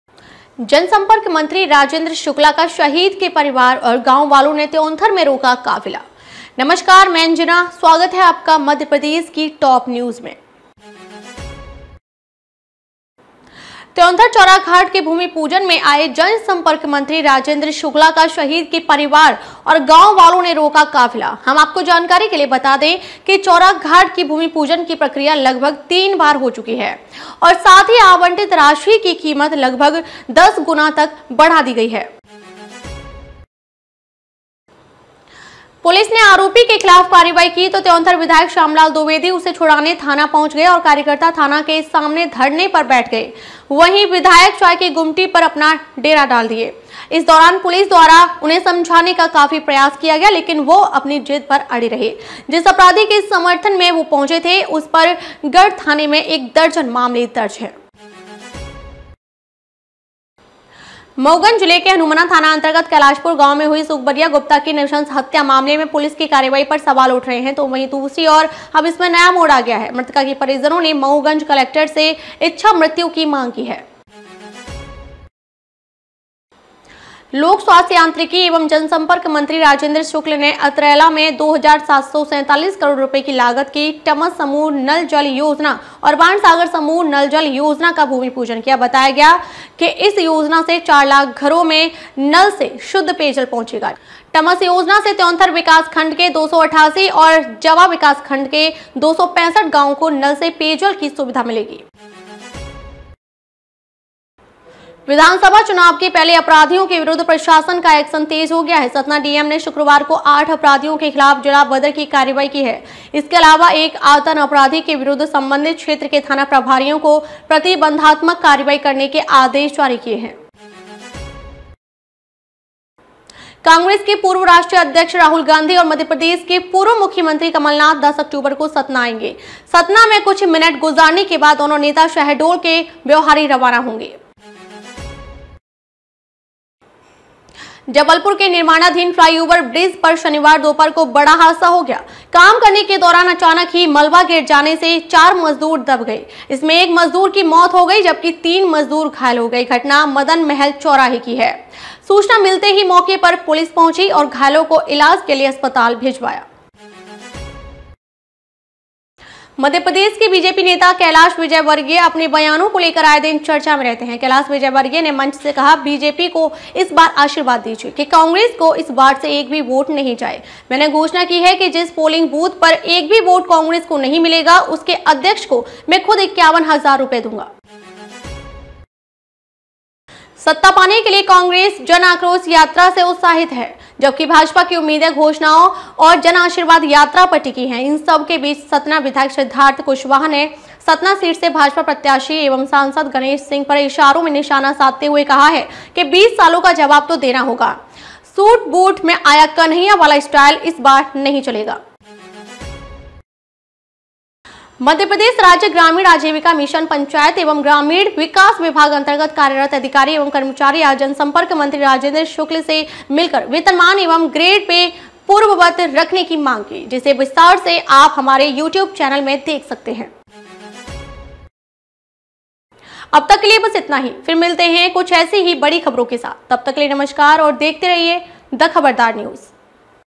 Today's TOP Headlines। आज की सुर्खियां । Rewa News। MP Latest News.
MP News: त्यौंथर चौराघाट के भूमि पूजन में आए  जन संपर्क मंत्री राजेंद्र शुक्ला का शहीद के परिवार और गांव वालों  ने रोका काफिला, हम आपको जानकारी के लिए बता दे की चौराघाट की भूमि पूजन की प्रक्रिया लगभग तीन बार हो चुकी है और साथ ही आवंटित राशि की कीमत लगभग 10 गुना तक बढ़ा दी गई है ।
पुलिस ने आरोपी के खिलाफ कार्यवाही की तो, त्योंथर विधायक श्यामलाल दुवेदी उसे छुड़ाने थाना पहुंच गए और कार्यकर्ता थाना के सामने धरने पर बैठ गए वहीं विधायक चाय की गुमटी पर अपना डेरा डाल दिए। इस दौरान पुलिस द्वारा उन्हे समझाने का काफी प्रयास किया गया लेकिन वो अपनी जिद पड़ अड़े रहे, जिस अपराधी के समर्थन में वो पहुंचे थे उस पर गढ़ थाने में एक दर्जन मामले दर्ज हैं। मऊगंज जिले के हनुमना थाना अंतर्गत कैलाशपुर गांव में हुई सुकबरिया गुप्ता की नृशंस हत्या मामले में पुलिस की कार्यवाही पर सवाल उठ रहे हैं तो वहीं दूसरी ओर अब इसमें नया मोड़ आ गया है मृतिका के परिजनों ने मऊगंज कलेक्टर से इच्छामृत्यु की माग की है।